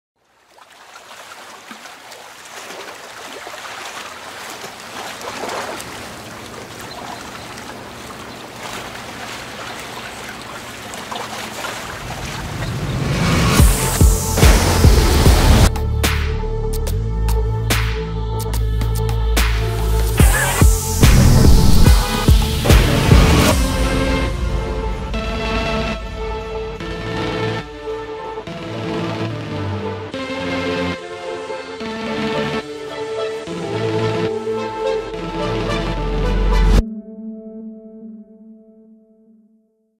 Anchored Trailer
The pull of culture's polarization, idolatry, and secularism seems to be getting stronger by the day. But, would it surprise you to know that this cultural moment we're living in isn't new? These waves of wickedness that we see on the streets of our cities have been infiltrating humanity since the Garden of Eden. We're going to look into the life of Elijah, an Old Testament prophet, and see how he stood strong for the things of God in a society that raged against righteousness.